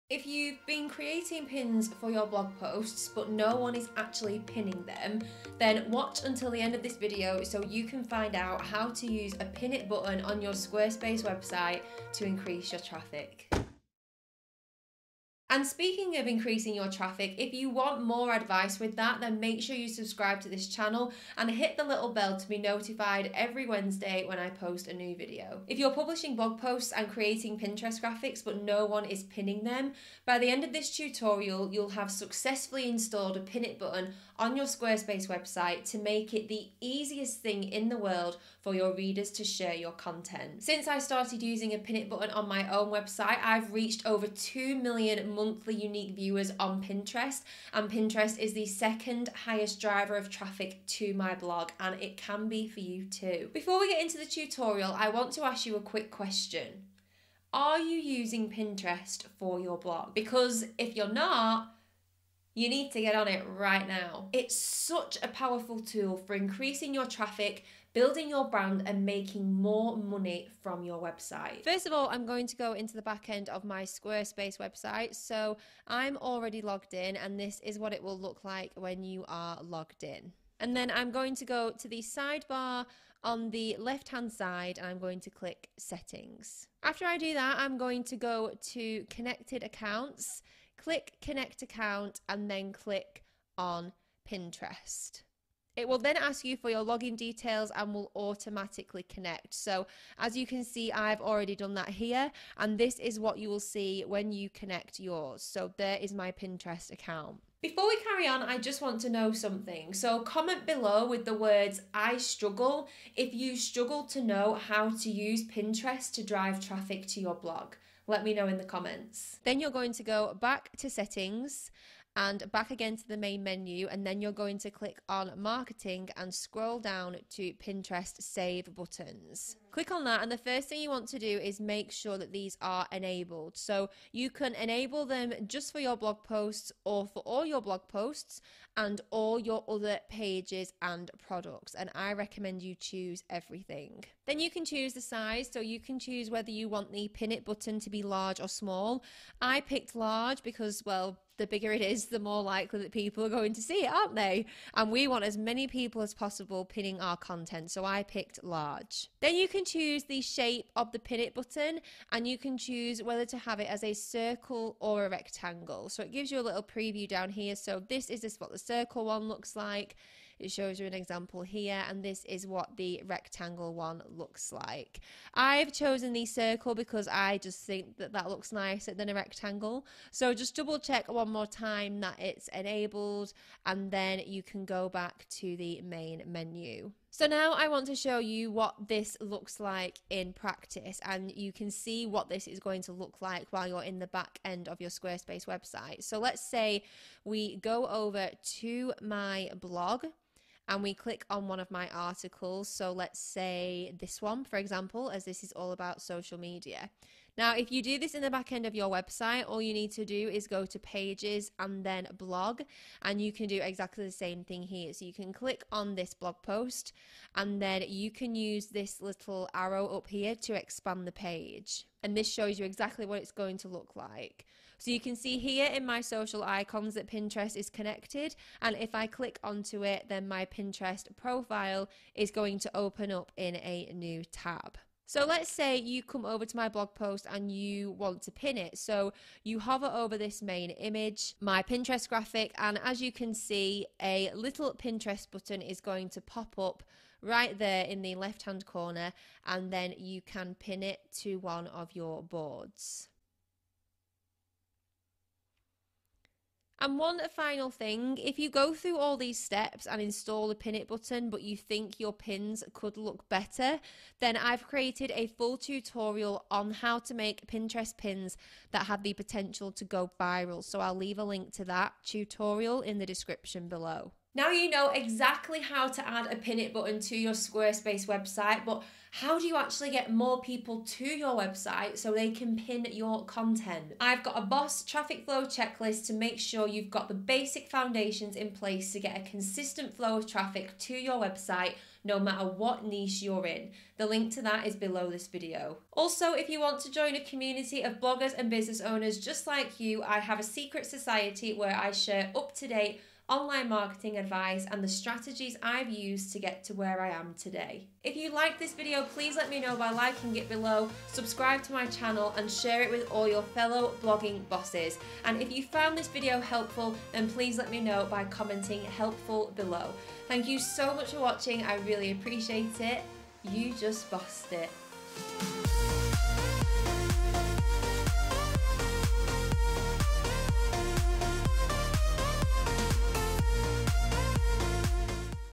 PINTEREST PIN IT BUTTON SQUARESPACE: HOW TO USE ONE TO INCREASE TRAFFIC
PINTEREST PIN IT BUTTON SQUARESPACE: HOW TO USE ONE TO INCREASE TRAFFIC // Want to add a pinterest pin it button to your website? This pinterest pin it button squarespace tutorial will show you how to create a pinterest pin it button for images and all of the best advice for creating pinterest pins. If you’re looking for ideas on the perfect pinterest pin size, or how a pinterest pin it button will look, and you feel like your pinterest pins aren’t cutting it, this video will show you everything you need to know to add a button to a pinterest pin.

Next time you’re looking to add a pin it button to your website, you’ll be set up with the best pinterest pin it button squarespace tutorial.